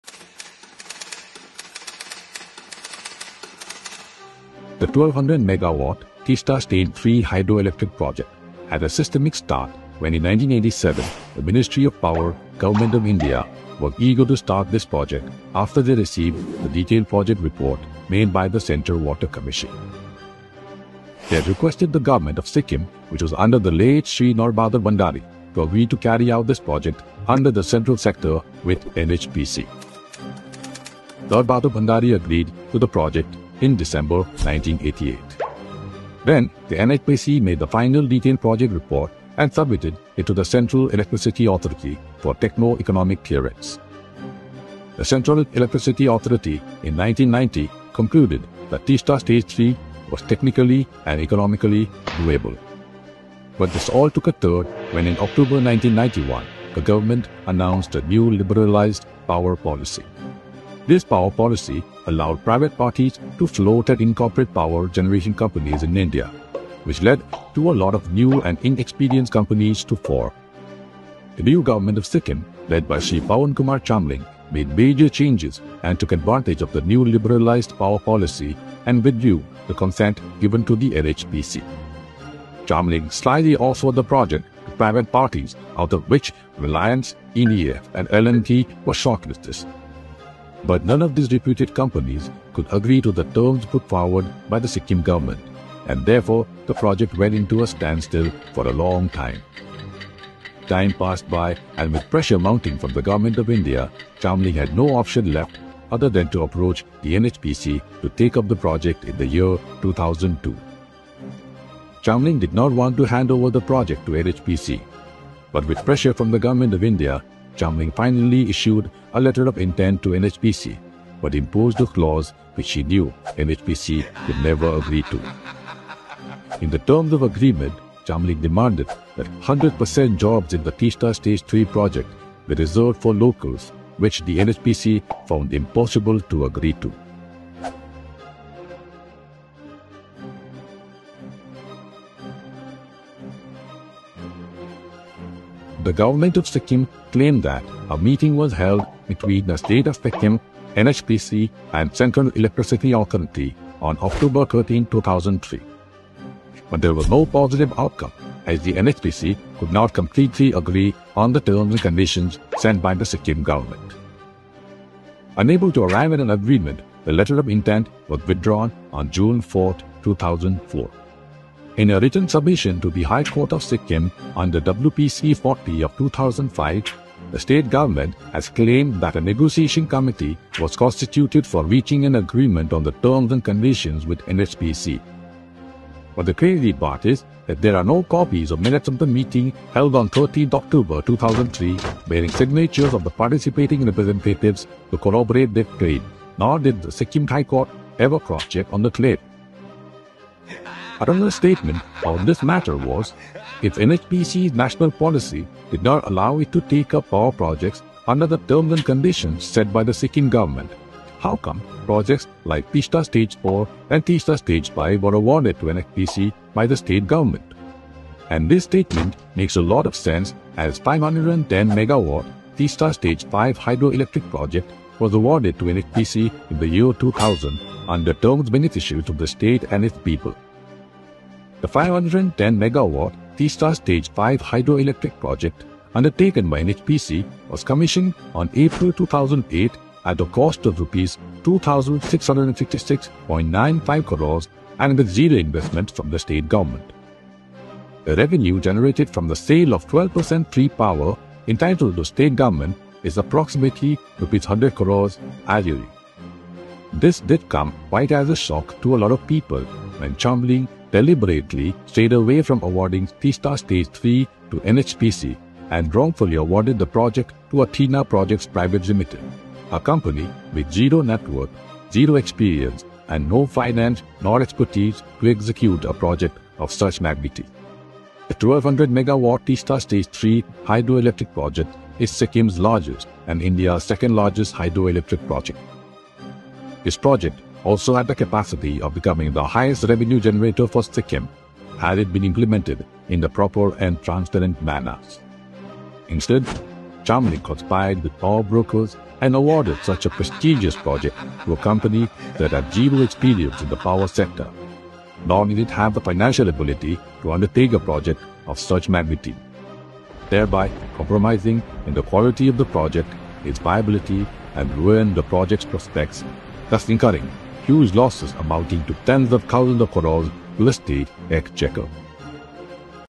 Sikkim Hydel Power Scam Under Pawan Chamling - Part 4
FACTS ABOUT THE 1200 MW TEESTA STAGE III HYDRO ELECTRIC PROJECT

The Centre Water Commission had after thorough investigation and survey, prepared a Detailed Project Report of 1200 MW Teesta Stage III Hydro Electric Project in the year 1987. The Ministry of Power, Govt of India requested the concurrence of Govt of Sikkim to carry out the 1200 MW Teesta Stage III Hydro Electric Project under the Central Sector with NHPC. The Govt of Sikkim, under Late Shri Nar Bahadur Bhandari, accorded concurrence to the 1200 MW Teesta Stage III Hydro Electric Project in 1988. The NHPC thereafter prepared a final Detailed Project Report (DPR) and submitted the same to the Central Electricity Authority (CEA) for Techno Economic Clearance. In December, 1990 the CEA concluded that the 1200 MW Teesta Stage III Hydro Electric Project was technically and economically viable.
Meanwhile the Govt of India announced a new liberalized power policy in October, 1991, permitting private parties to float and incorporate power generation companies in India. The state had also witnessed a change with Shri Pawan Kumar Chamling led SDF forming the new government in 1994. Taking advantage of the new liberalised power policy of the union of India, the Govt of Sikkim under Pawan Kumar Chamling, withdrew the previous consent to NHPC and in August, 1996, solicited offers from private parties for the development of 1200 MW Teesta Stage III Hydro Electric Project. Ultimately 3 parties namely, Reliance, EDF and L & T were shortlisted but none were awarded the project, which remained in a standstill for a long time.

Again in the year 2002, the Govt of Sikkim approached the NHPC to pursue the 1200 MW Teesta Stage III Hydro Electric Project and issued a Letter of Intent (LoI) dated 2nd December 2002, subject to acceptance of the terms and conditions set by the Sikkim government. In a written submission to the High Court of Sikkim under WP (C) 40 of 2005, the state government has claimed that a negotiation committee was constituted for reaching an agreement on the terms and conditions with NHPC. Furthermore, it submits that on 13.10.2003, the Additional Secretary, Ministry of Power, Govt of India conveyed a joint meeting between State of Sikkim, NHPC and Central Electricity Authority. However, the meeting had no positive outcome as NHPC did not agree to the terms and conditions of the state govt, citing that it had a set of guidelines for the entire country and could not deviate from it. Unable to arrive at an agreement, as claimed by the Sikkim Energy and Power Department, the Letter of Intent issued to NHPC for 1200 MW Teesta Stage III Hydro Electric Project was therefore withdrawn on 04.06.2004. However, all of these claims were solely based on a letter issued by the Energy and Power Department, Govt of Sikkim. There were no copies of minutes of the meeting held on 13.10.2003, bearing signatures of the participating representatives, to corroborate their claims nor did the Sikkim High Court ever cross check on the claim. The other contention is that, “if NHPC’s national policy did not allow it to take up power projects under the terms and conditions of the Sikkim government, how come projects Teesta Stage IV and Teesta Stage V were awarded to NHPC by the state government.”

In fact, 510 MW Teesta Stage V Hydro Electric Project was awarded to NHPC in the year 2000, under terms beneficial to the state and its people. The 510 MW Teesta Stage V Hydro Electric Project, undertaken by NHPC (a Govt of India Enterprise} was commissioned on April 2008 (after 13-months delay) at a cost of Rs 2656.95 cr and with zero investment from the state government. The revenue generated from the sale of 12% free power entitled to state govt is approximately Rs. 100 cr, annually.

However, it is unfortunate that the then Chief Minister Shri Pawan Kumar Chamling deliberately strayed away from awarding the prestigious 1200 MW Teesta Stage III Hydro Electric Project to NHPC and wrongfully awarded the project to M/S Athena Projects Pvt Ltd, a company with zero net worth, zero experience and no finance nor expertise to execute a project of such magnitude.

The 1200 MW Teesta Stage III Hydro Electric Project is Sikkim’s largest and India’s second largest Hydro Electric Project. This project also had the capacity of becoming the highest revenue generator for Sikkim, had it been implemented in a proper and transparent manner. Instead the then Chief Minister (also Minister in-charge of Energy and Power Department) Shri Pawan Kumar Chamling conspired with power brokers and awarded such a prestigious project to a company, that had zero experience in the power sector nor did it have the financial ability to undertake a project of such magnitude. Thereby, compromising in the quality of the project, its viability and ruin the project’s prospects, thus incurring losses amounting to tens of thousands of crores to the state exchequer